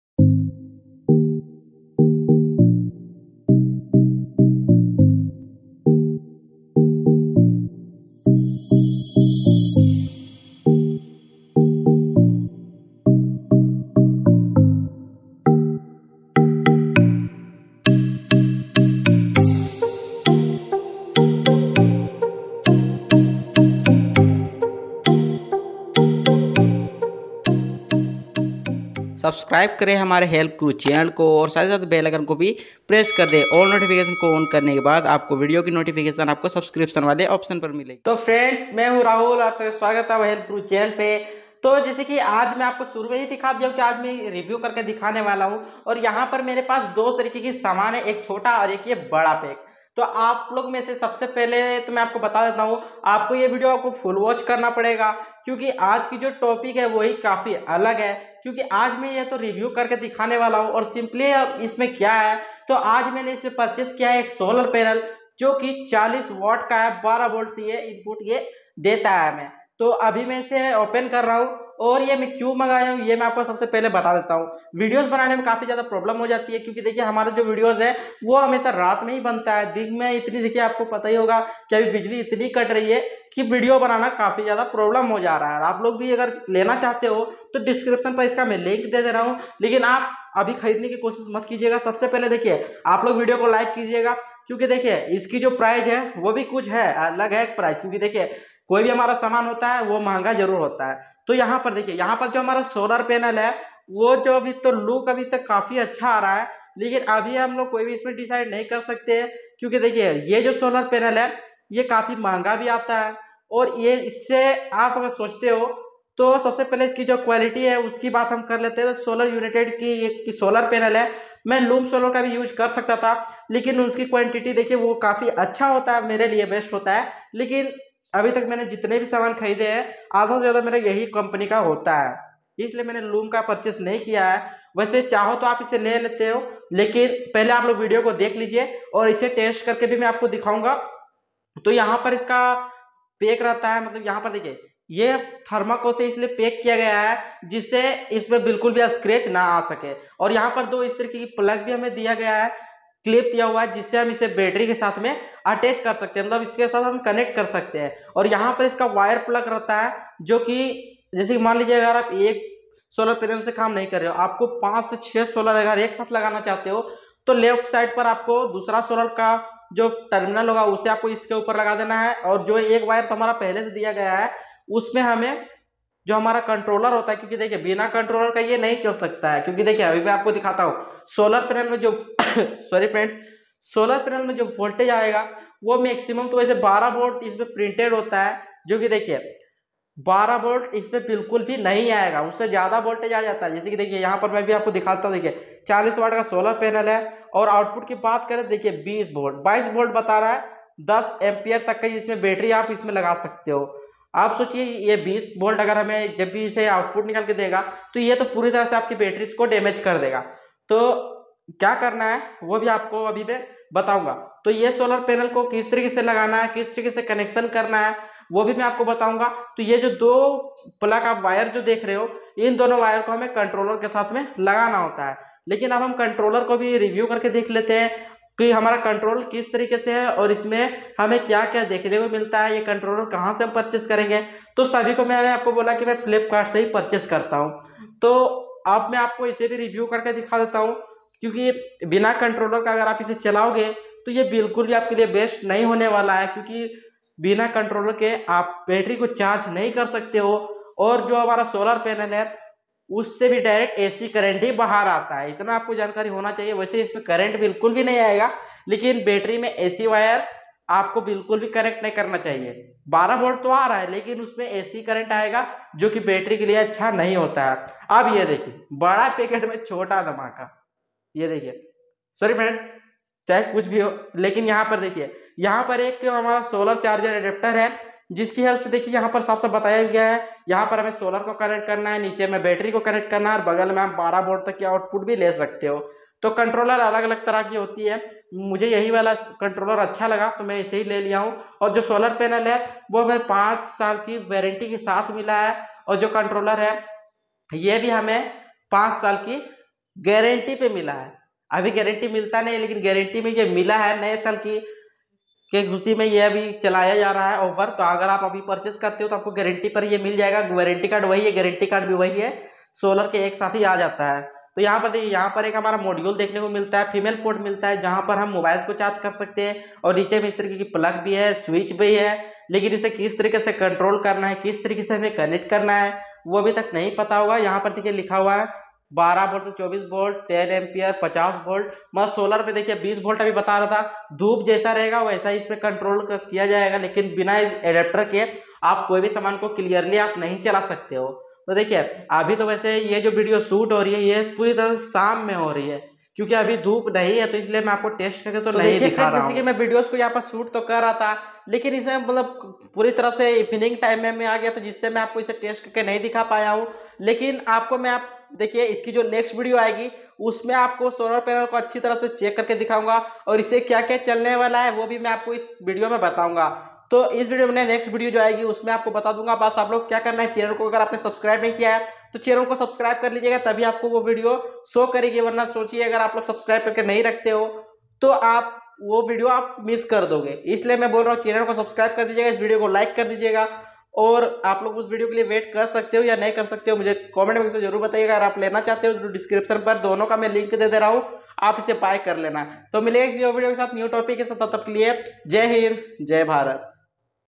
12volt 40watt Solar Panel Review|Solar Panel Unboxing In Hindi|40watt Solar Review Full Review Video
Hello Friends,

So This Video Topic Is A Small 40watt Solar Panel Review Video In Hindi.